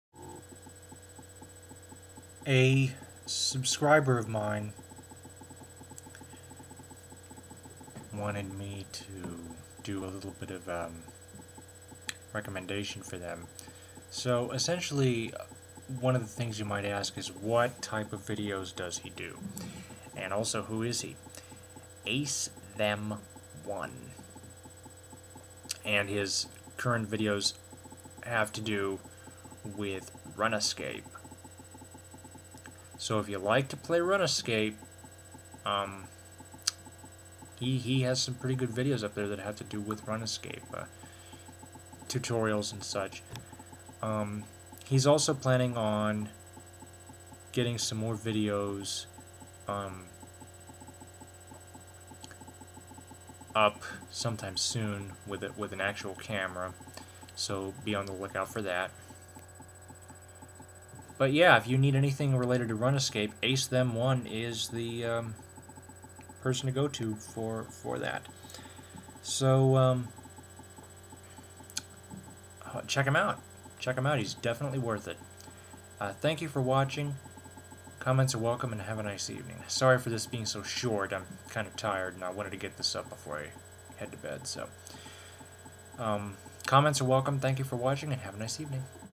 A Subscriber's Channel that I Recommend
The title says it all. Check out AceThem1 for anything related to Runescape. :)